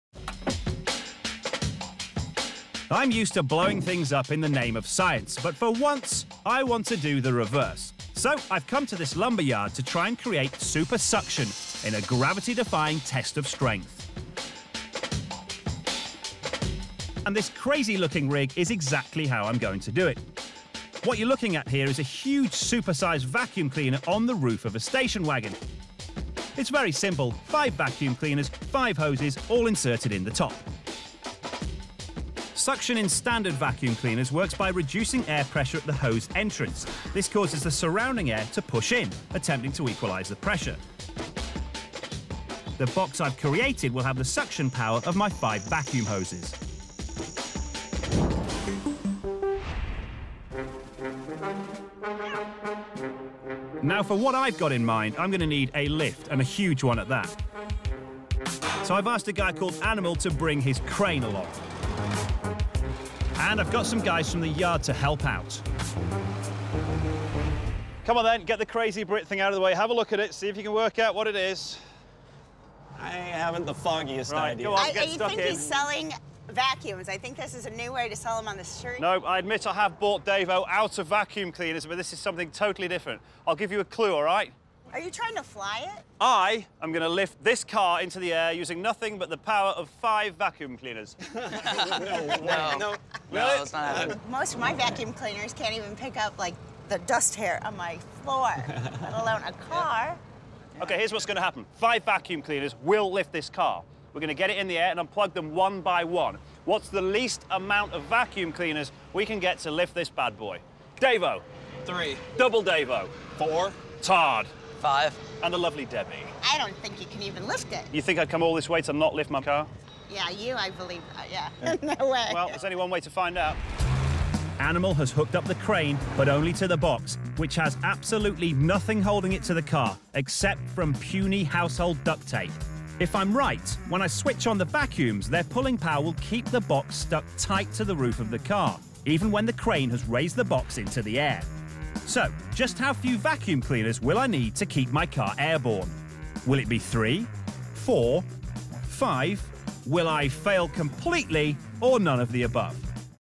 Super Suction | National Geographic
Tim explores how many vacuum cleaners it takes to hold up a station wagon.